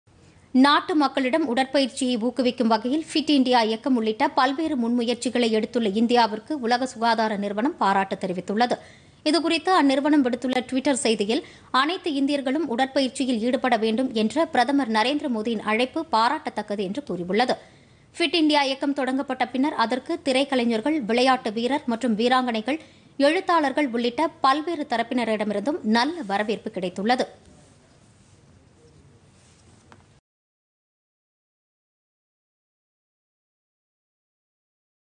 உலக சுகாதார நிறுவனம் பாராட்டு
இந்தியாவில் உடற்பயிற்சியை ஊக்குவிக்க மத்திய அரசு  மேற்கொண்டு வரும் முயற்சிகளுக்கு உலக சுகாதார நிறுவனம் பாராட்டு